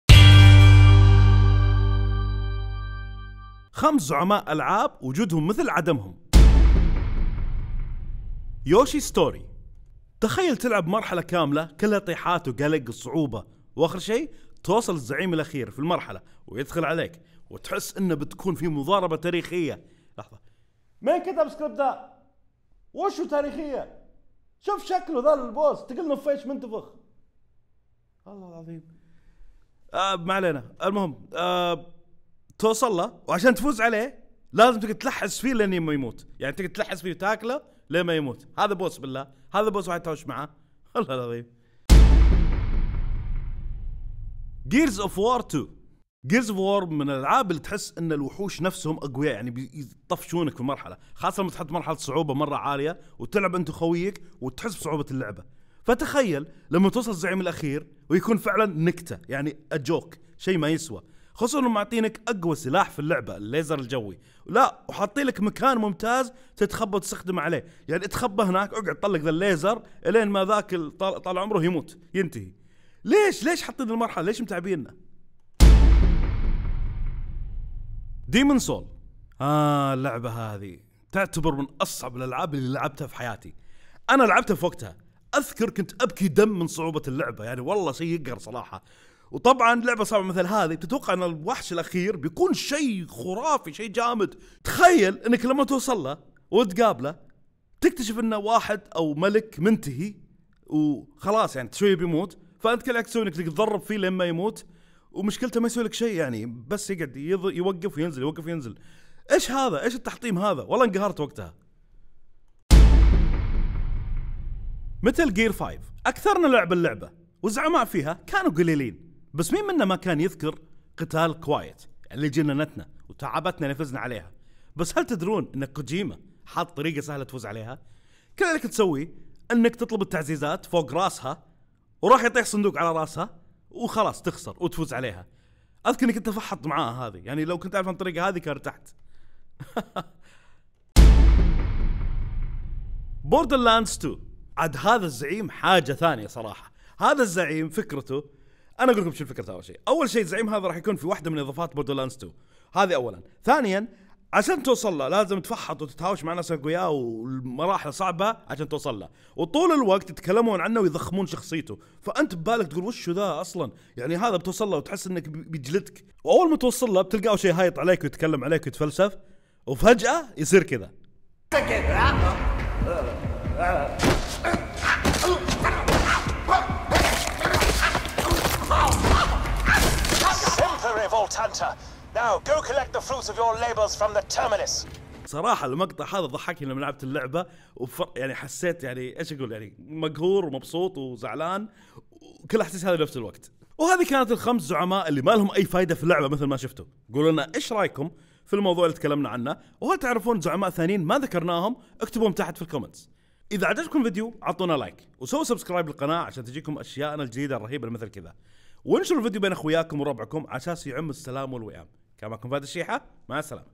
5 زعماء مامنهم فايدة في الألعاب
الزعيم يعتبر أساس اللعبة وهو الي يحرك القصة للأمام وكثير من الاوقات يكون قوي جدا ولكن في بعض الالعاب الزعيم يكون عبارة عن اضحوكة من سهولته

محمد البسيمي @albsimi
فهد الشيحة @falshi777a
أحمد المصطفى @ahmedroid